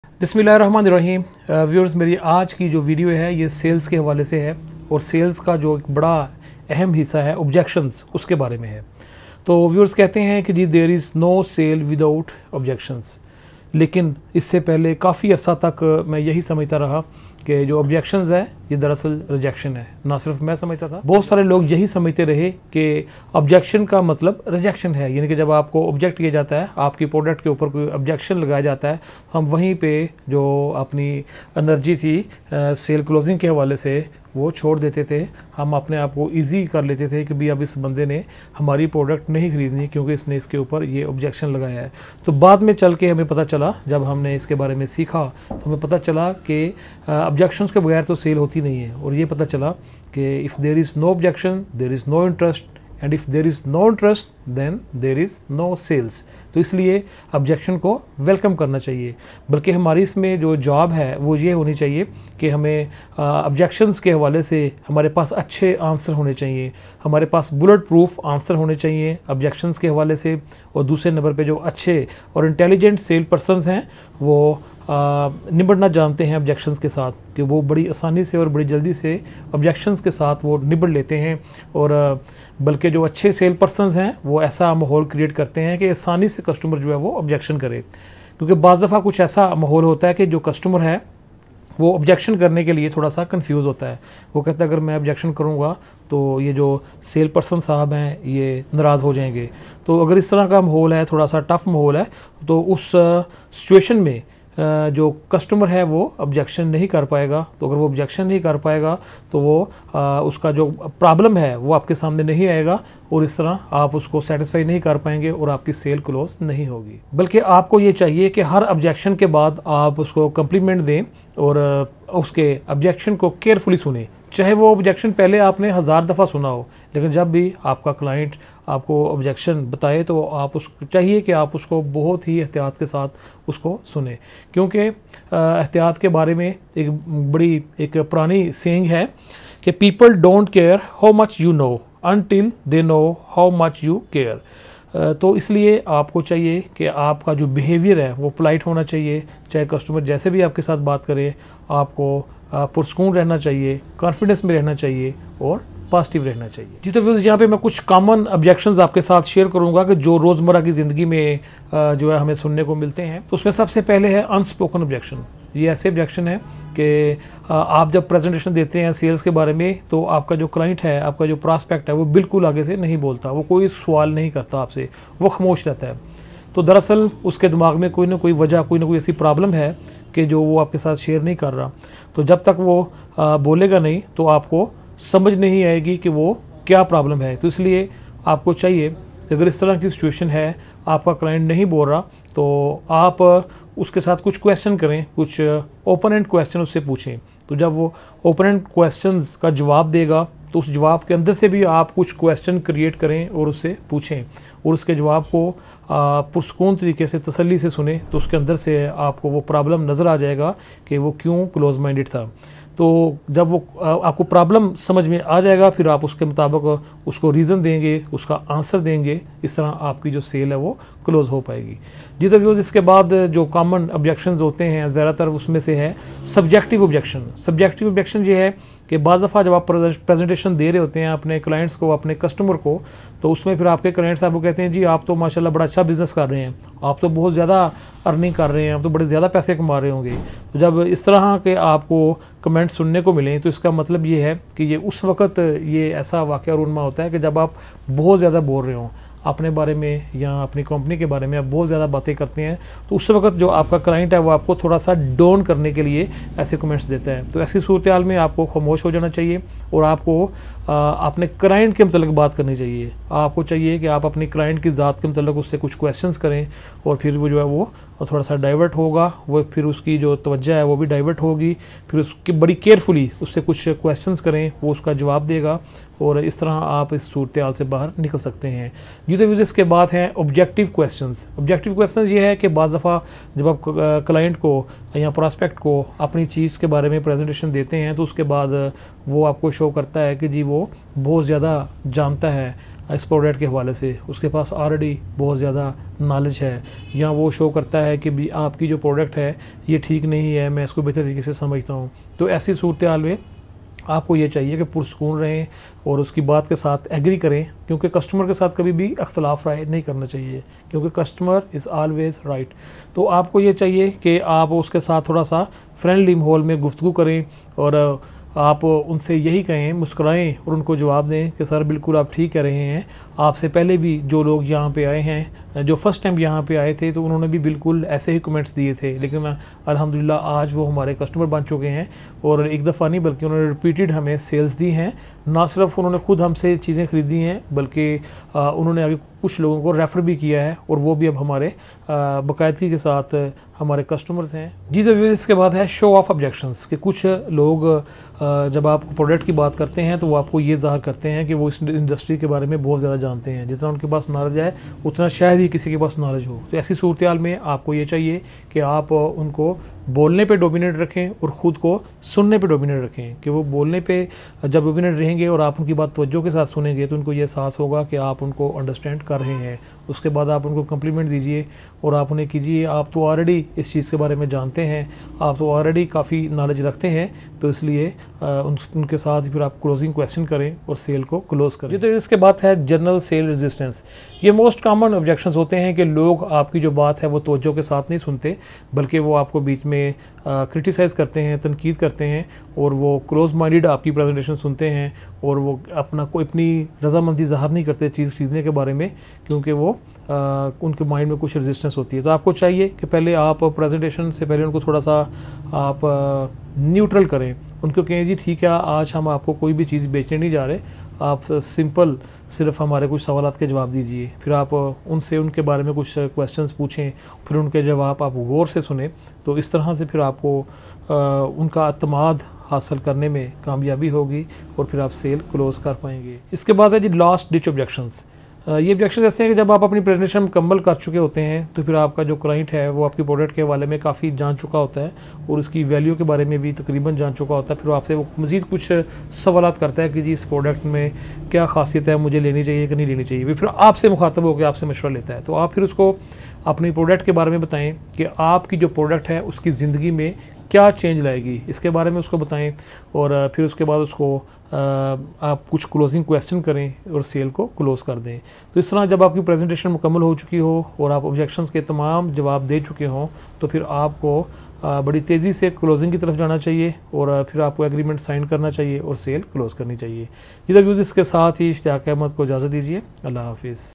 How to handle any objection in sales | Learn Sale by Rana Ishtiaq Ahmed
Learn Sales by Rana Ishtiaq Ahmed
In This video we will learn the importance of objections in  sale, and how to tackle any situation in sales.